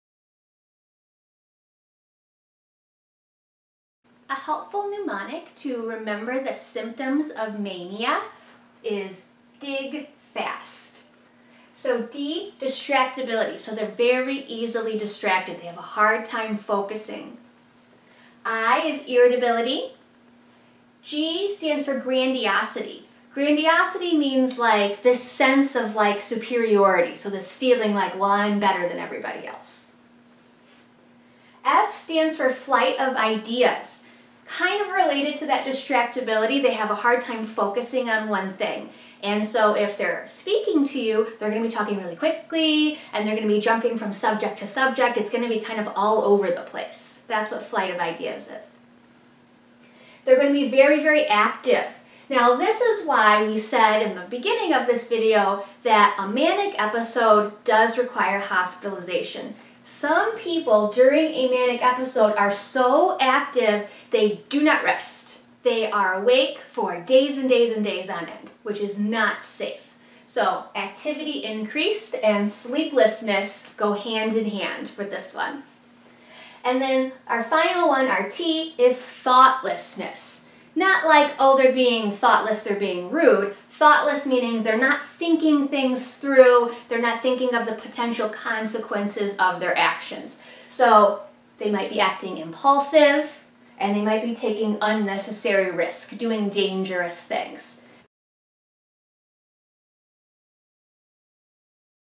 MNEMONIC BIPOLAR MANIA
In this video I breakdown a mnemonic device used to identify the signs and symptoms of Bipolar Mania.